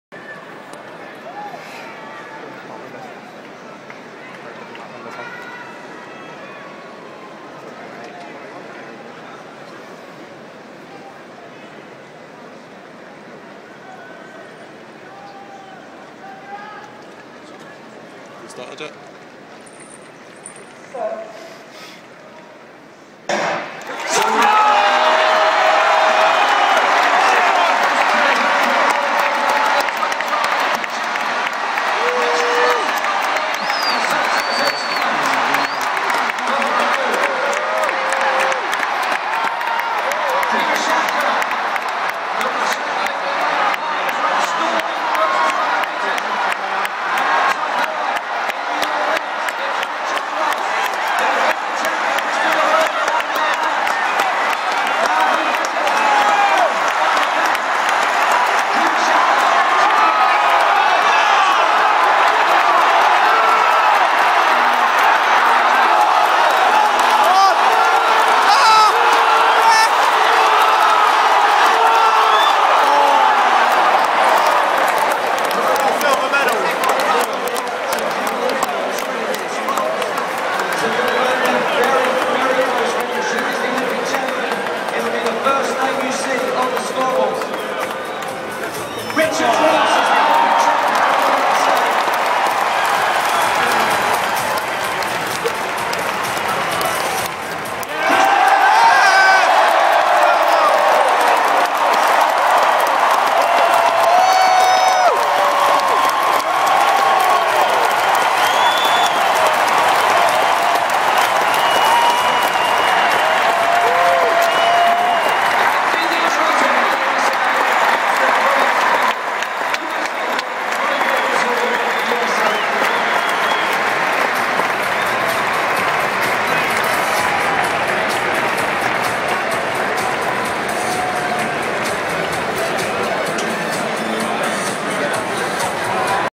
Women's 400m Final - London 2012
Participants   React. Result
1 Sanya Richards-Ross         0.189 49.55  USA
2 Christine Ohuruogu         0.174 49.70  GBR
3 Deedee Trotter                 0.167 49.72  USA
4 Amantle Montsho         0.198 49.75  BOT
5 Novlene Williams-Mills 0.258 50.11  JAM
6 Antonina Krivoshapka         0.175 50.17  RUS
7 Francena Mccorory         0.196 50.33  USA
8 Rosemarie Whyte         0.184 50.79  JAM